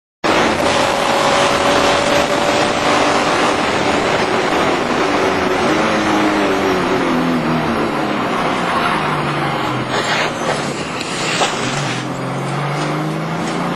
Porsche Crash
One of them literally drives over the other.